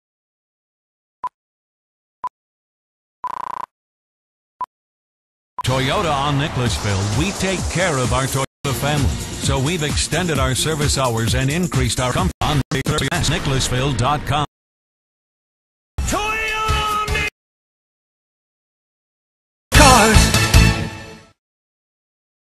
Toyota's new commercial! With a new car that changes the game
where price sells cars.

{school safe}

The ..idea.. was derived from the ..Arbys guy.. on my channel.